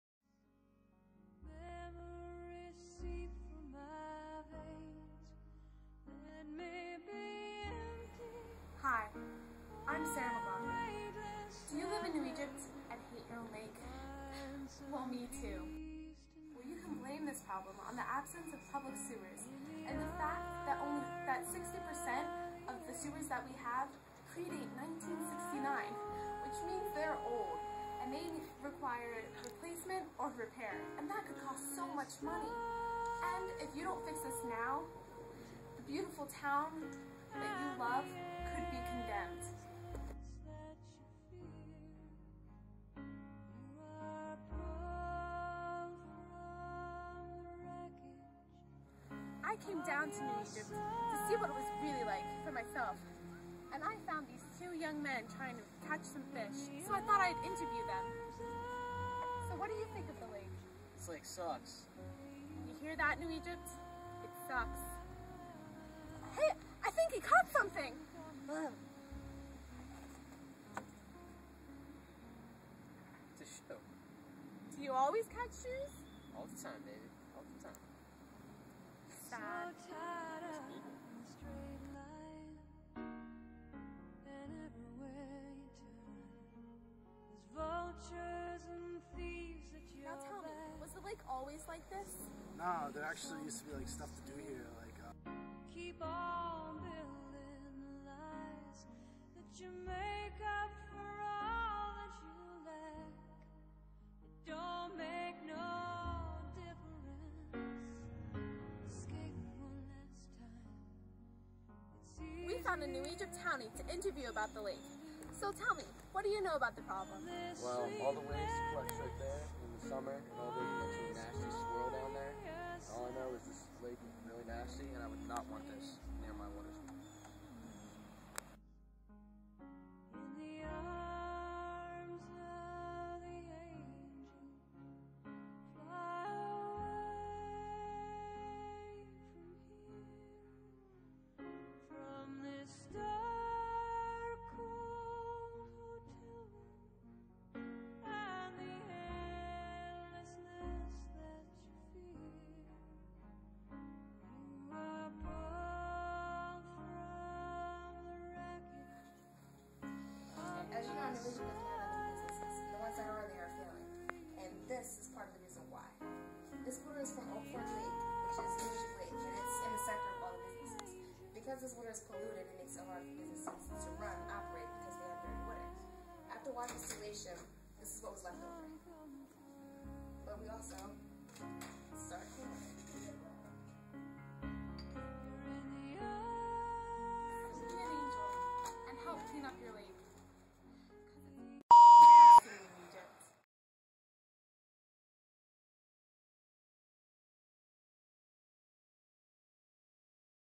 New Egypt Lake
Informative video about the New Egypt Lake created by students